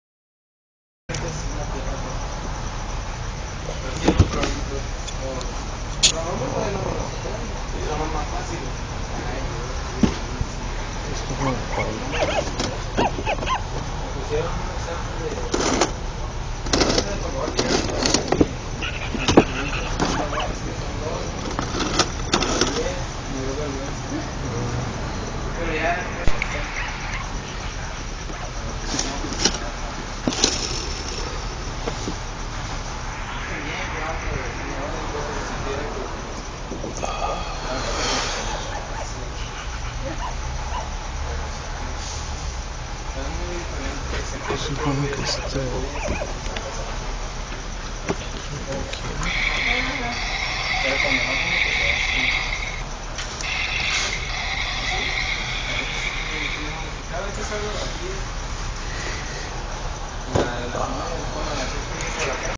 Faltan Transformers para Navidad
Algunas figuras llegadas a tiendas Chedraui aquí en la ciudad. Optimus y Bumblebee no me llamaron la atención pero si el Optimus Maximus... si tuviera figuras cyberverse me canso que si lo compro! ^^